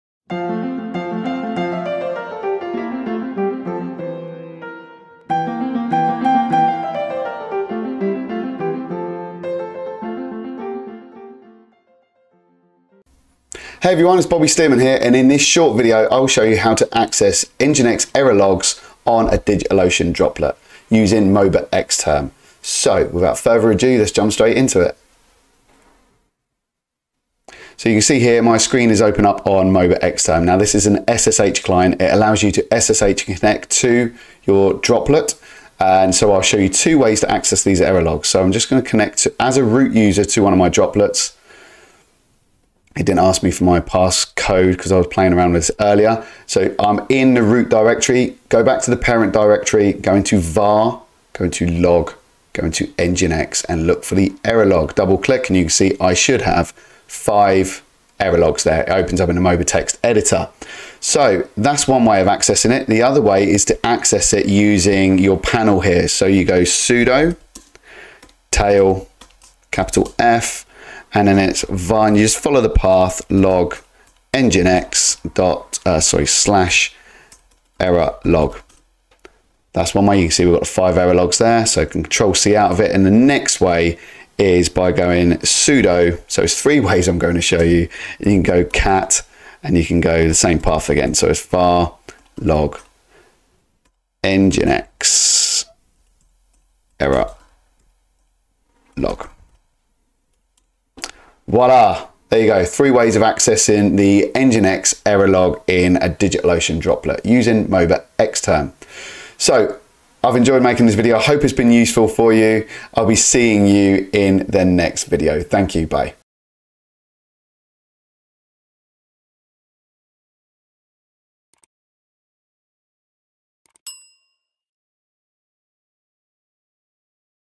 How To Access Nginx Error Logs | Ngnix Tutorial
In this programming Nginx tutorial, you will learn to access Nginx error logs.

NGINX is open-source software for web serving, reverse proxying, caching, load balancing, media streaming, and more.
It started out as a web server designed for maximum performance and stability. In addition to its HTTP server
capabilities, NGINX can also function as a proxy server for email (IMAP, POP3, and SMTP) and a reverse proxy and
load balancer for HTTP, TCP, and UDP servers.

When using Nginx in DigitalOcean ic can be very useful to be able to access the error logs file to help debug your
application. In this video, I will show you 3 ways you can assess the Nginx error.log file.